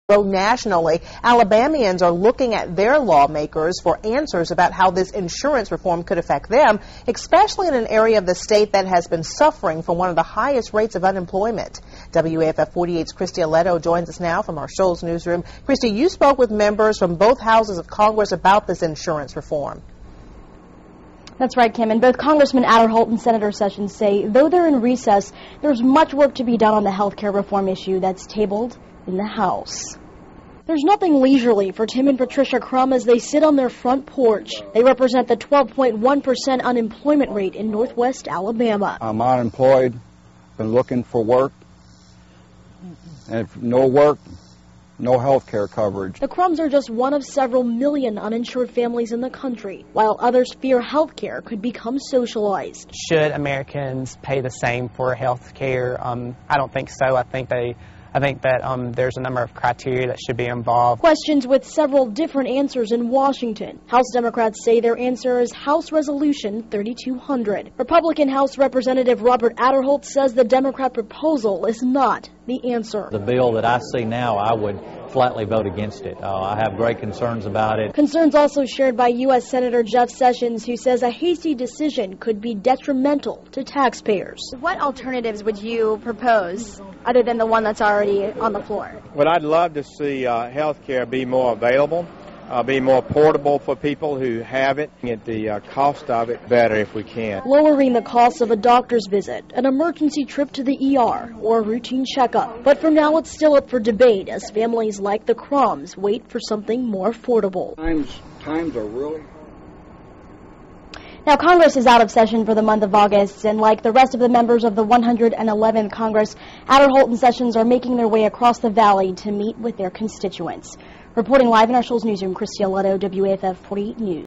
WAFF48 News: Aderholt opposes Healthcare proposal
FLORENCE, AL (WAFF)- August 11, 2009 -- As discussions grow nationally, Alabamians are looking to their lawmakers for answers about how health care reform could affect them.

WAFF 48 News spoke with members from both houses of congress about the health care reform on Tuesday in Northwest Alabama, where the unemployment rate is one of the highest in the state.

Both Congressman Aderholt and Senator Sessions said that even though they're in recess, there's much work to be done on the health care reform issue tabled in the U.S. House of Representatives.